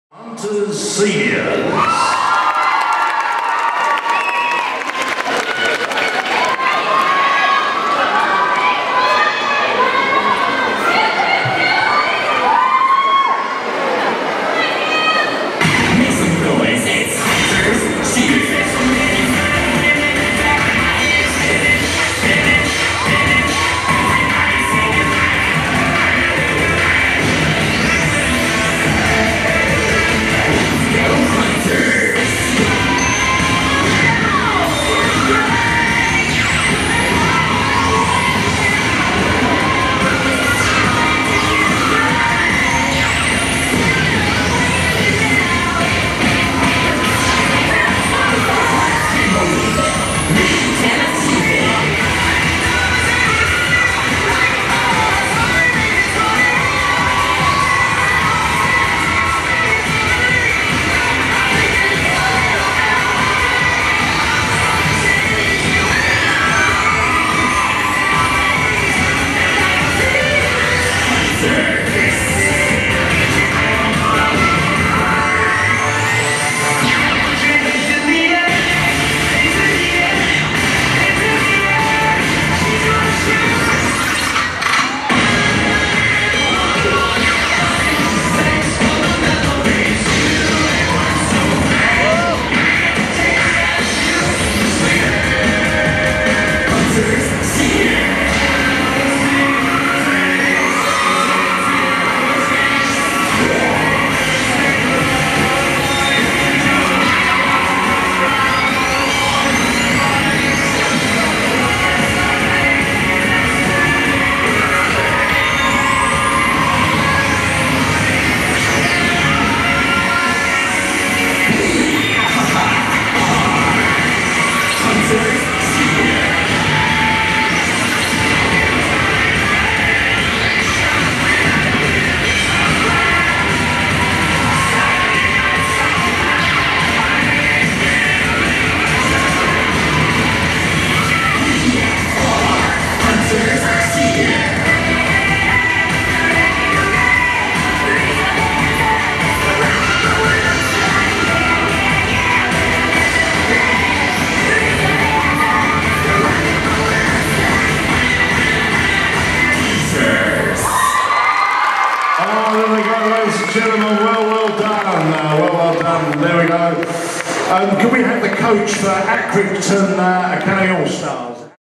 Future Cheer Jungle Fever 2014 - Horwich Hunters Senior 3
First time competing in cheer for over 2 years!

Videography Credit: Starshot